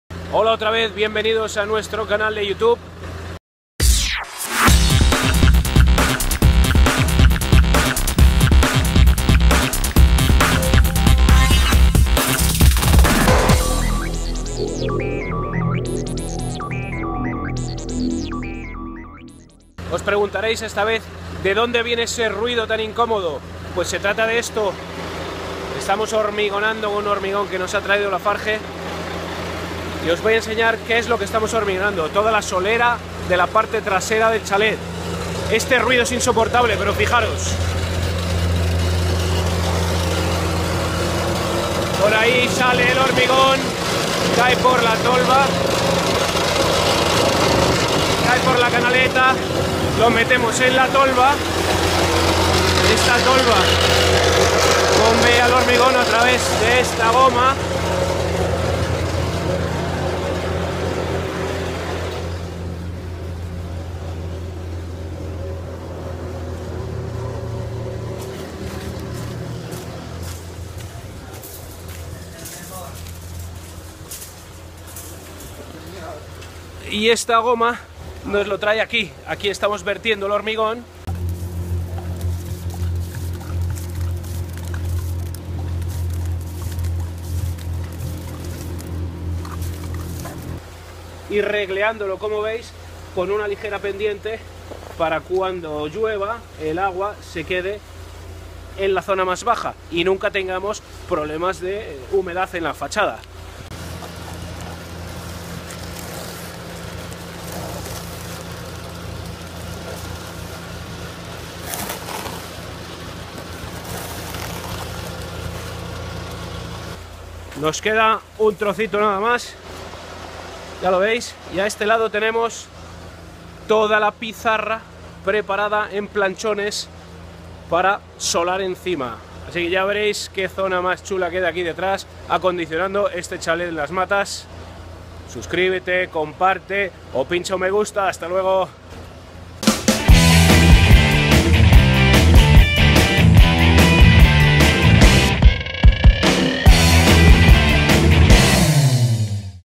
Reforma en Madrid (Las matas) - Parte 2
Hola amigos! En esta nueva entrega, David nos muestra cómo se bombea el hormigon hasta la parte trasera del chalet, y cómo se extiende para, posteriormente, colocar la pizarra. No os perdais la próxima entrega!